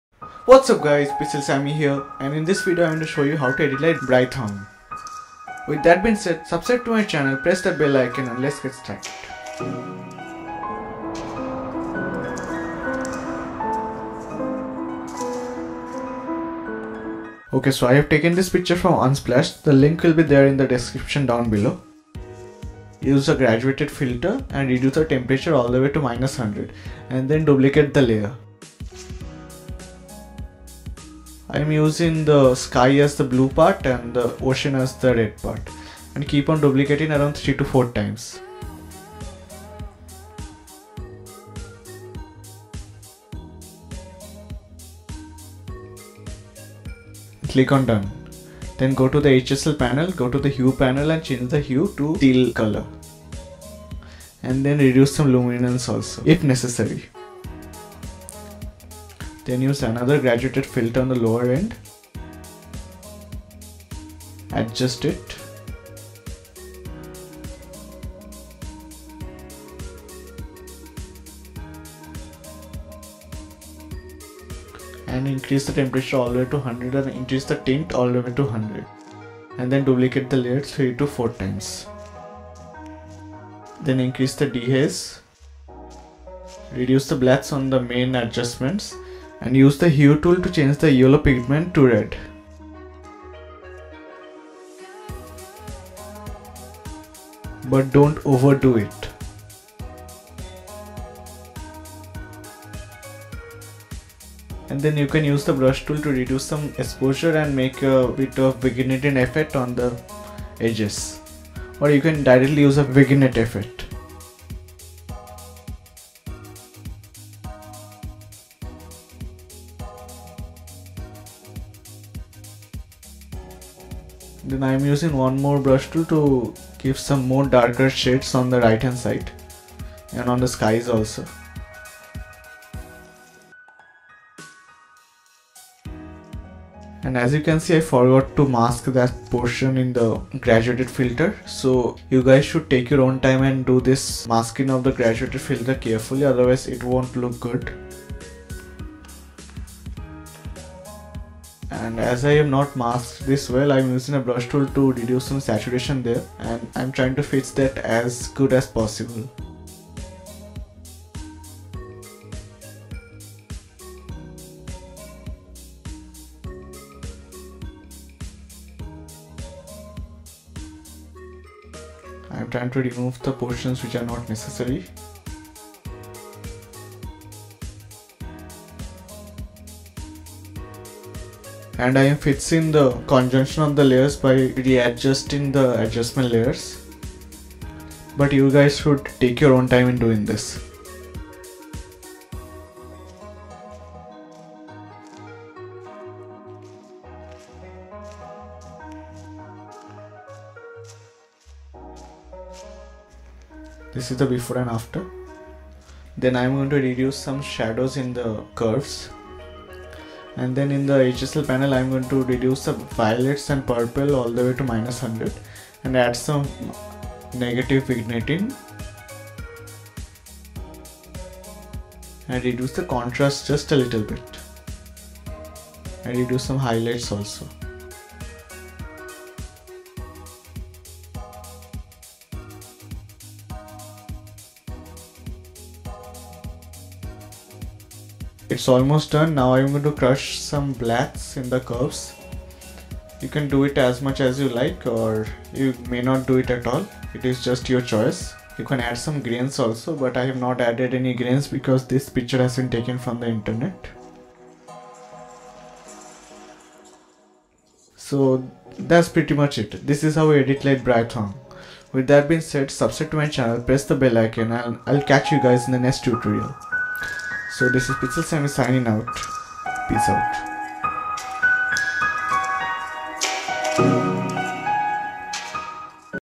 How to: Edit like Brightong \\ Lightroom
Hello Guys,
In this video, I'm going to show you guys how to edit like the famous Instagrammer and Photographer @brightong.

Software Used :
Final Cut Pro X